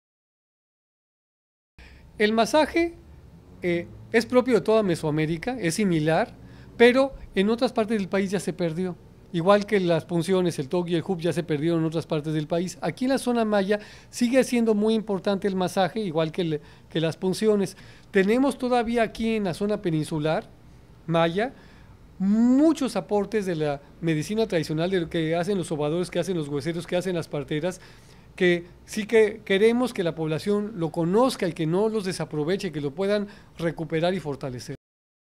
UADY TV 1/06/12 : Medicina Tradicional Maya
Entrevista con Hernán García Ramírez sobre la medicina tradicional maya en la peninsula de Yucatán.